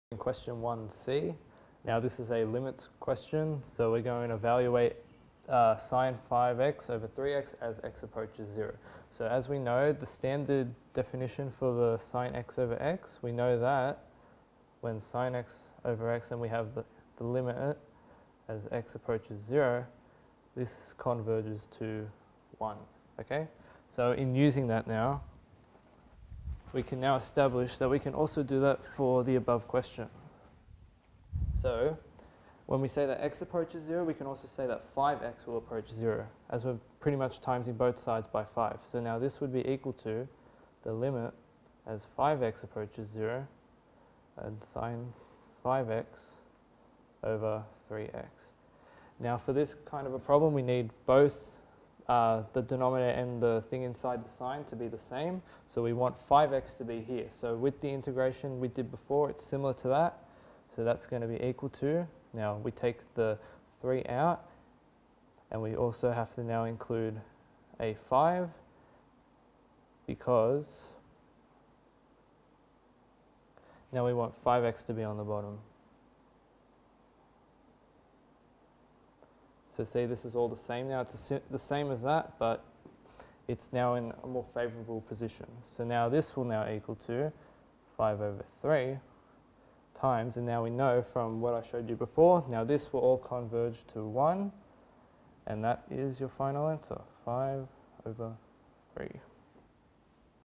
HSC 3U Mathematics 2006 Q1(c)
Presented by Damian Abood, in this series we'll go through the 3U Mathematics HSC Paper of 2006, question by question.